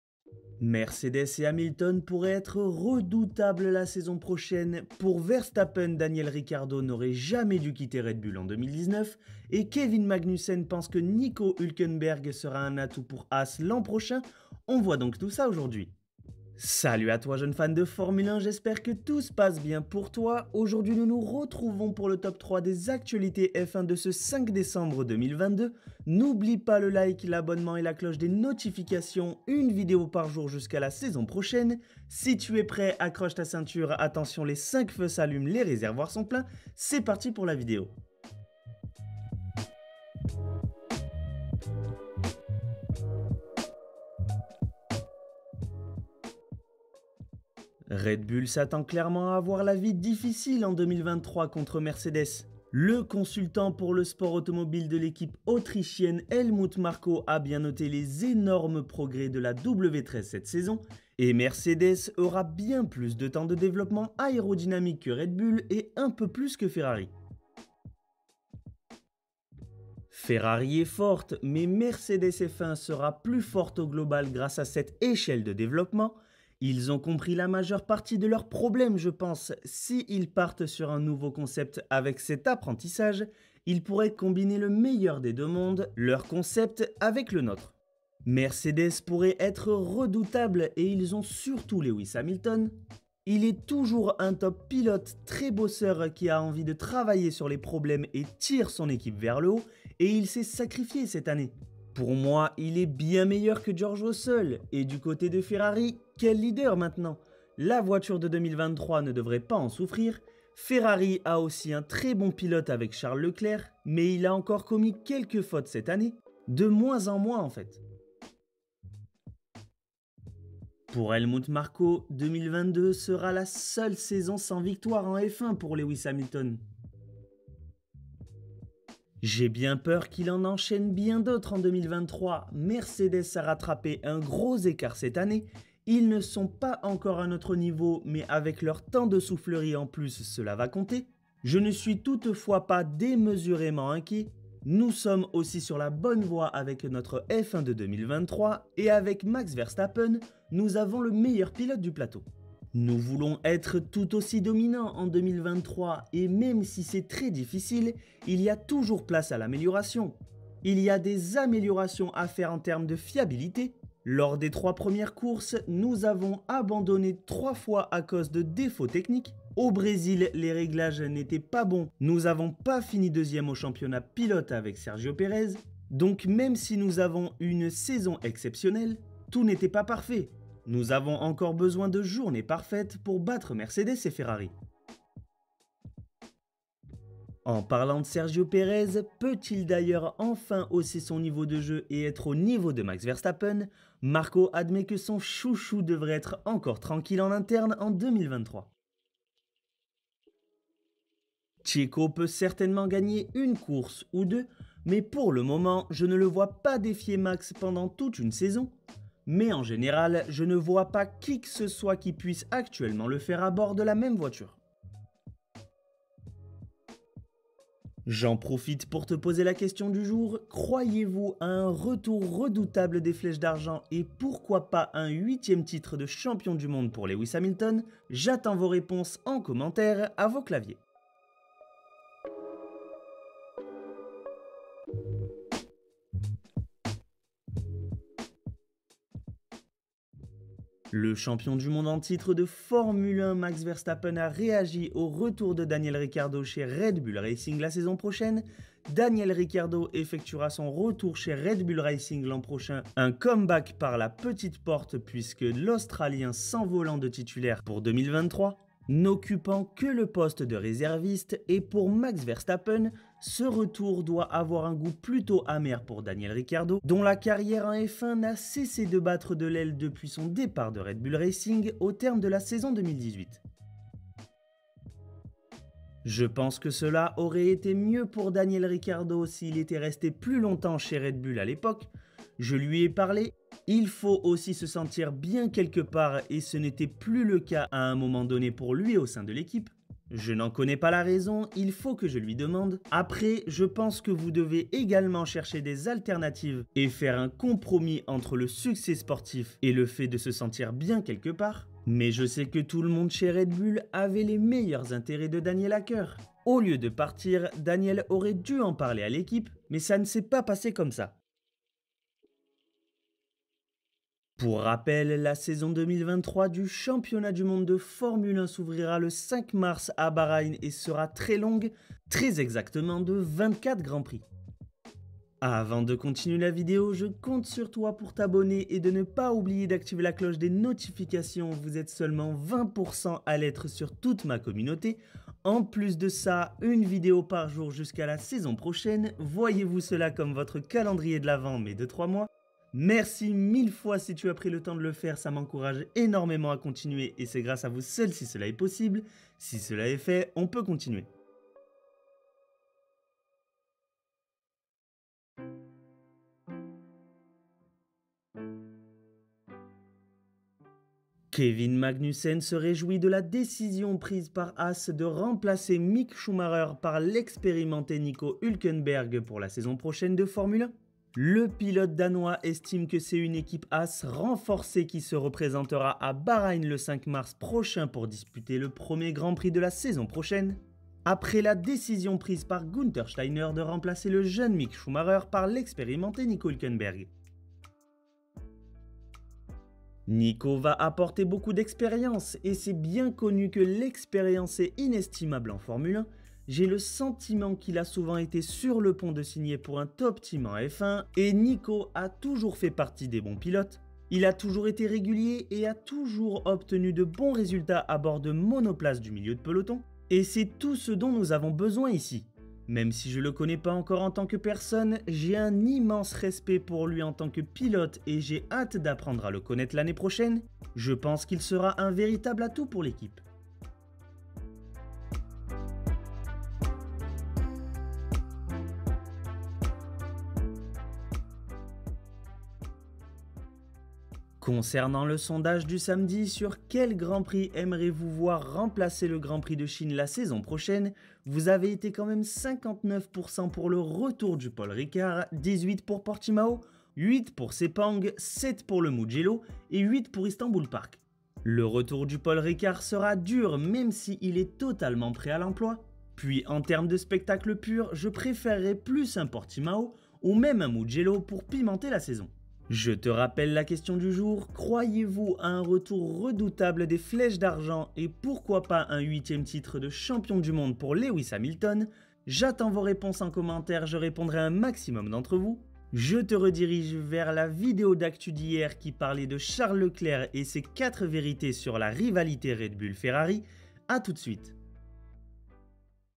ET SI MERCEDES ET LEWIS HAMILTON DÉCOUPAIT LA CONCURRENCE EN 2023 ?!
ET SI MERCEDES ET LEWIS HAMILTON DÉCOUPAIT LA CONCURRENCE EN 2023 ?! - LE PODIUM F1

00:00 introduction
00:39 Mercedes et Hamilton pourraient être redoutable la saison prochaine
03:52 Pour Verstappen Daniel Ricardo n'aurait jamais dû quitter Red Bull en 2019
05:52 Kevin Magnussen pense que Nico Hulkenberg sera un atout pour Haas l'an prochain

Salut à TOI ! Jeune fan de formule 1 !

Beaucoup d'actualité en formule 1 aujourd'hui ! Mercedes et Hamilton pourraient être redoutable la saison prochaine, pour Verstappen Daniel Ricardo n'aurait jamais dû quitter Red Bull en 2019 et Kevin Magnussen pense que Nico Hulkenberg sera un atout pour Haas l'an prochain, on voit donc tout ça aujourd'hui !

En tout cas, j'espère que tu apprécieras la vidéo !

LE PODIUM F1